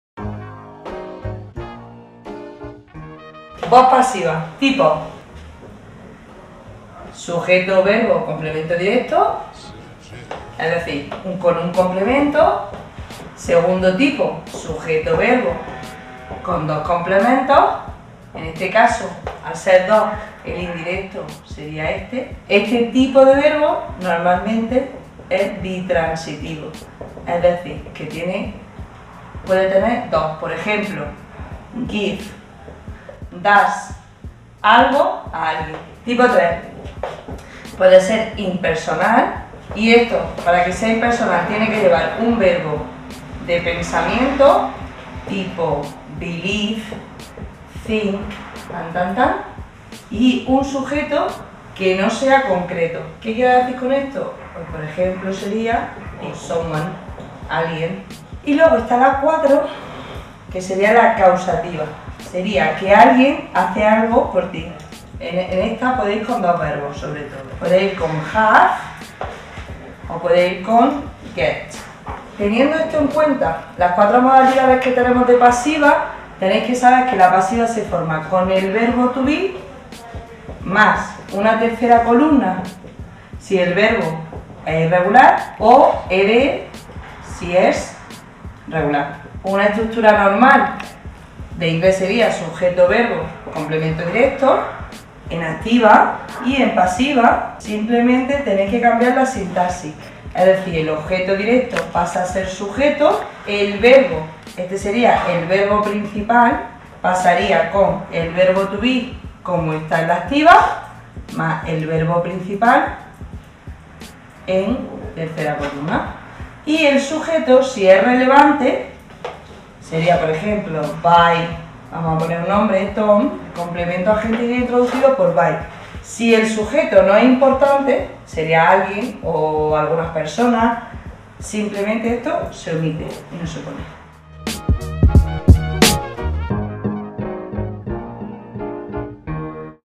Cómo formar frases pasivas en inglés - THE GLOBE FORMACIÓN
¿Sabes cuál es el uso de la voz pasiva en inglés? Nuestra docente Laura Rosario te explica detalladamente los cuatro tipos de frases pasivas y sus respectivos usos.